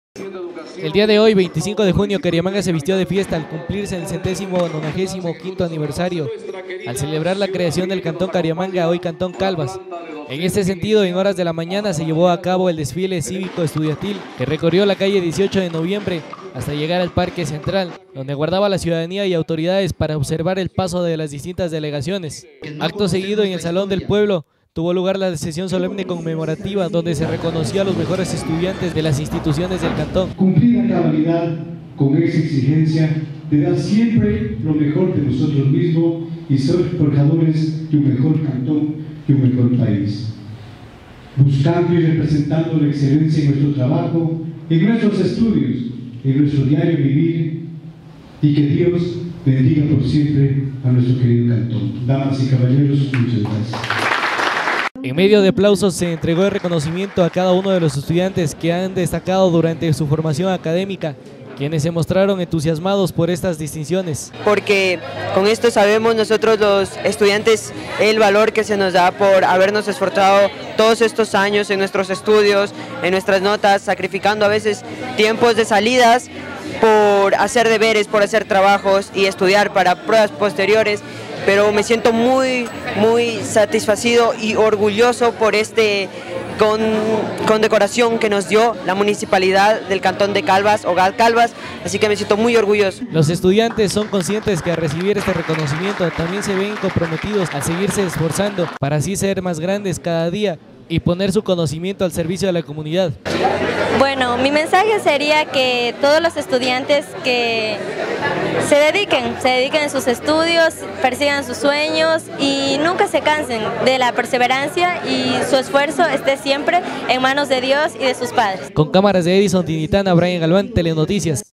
DESFILE CIVICO
Reconocimiento de parte las autoridades del municipio de calvas, durante la sesión solemne conmemorativa del 25 de junio